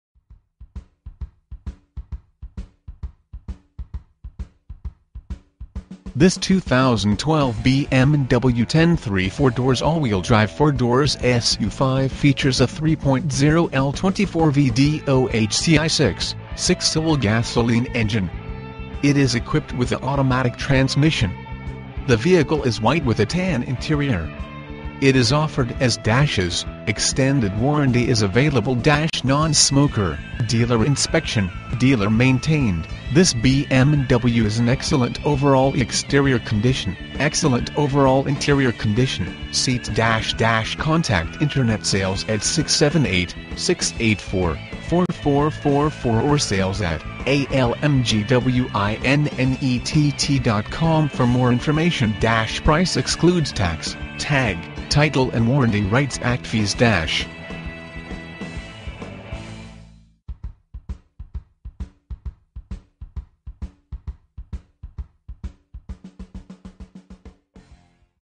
2012 BMW X3 - Atlanta Luxury Motors - Duluth, GA 30096
Atlanta Luxury Motors
2520 Pleasant Hill Road

For Sale: 2012 BMW X3
VIN: 5UXWX5C58CL719217
Engine: 3.0L 24V DOHC I6
Drivetrain: All Wheel Drive
Transmission: Automatic
Mileage: 17,180
Color: WHITE (exterior) Tan (interior)

Features:
This 2012 BMW X3 four doors all wheel drive four doors S U V features a 3.0L 24V DOHC I6 6cyl Gasoline engine. It is equipped with a Automatic transmission. The vehicle is WHITE with a Tan interior. It is offered As-Is, extended warranty is available. - Non-Smoker, Dealer inspection, Dealer maintained, This BMW is in Excellent overall exterior condition, Excellent overall interior condition, seats - - Contact Internet Sales at or for more information. - Price excludes tax, tag, title and Warranty Rights Act fees. -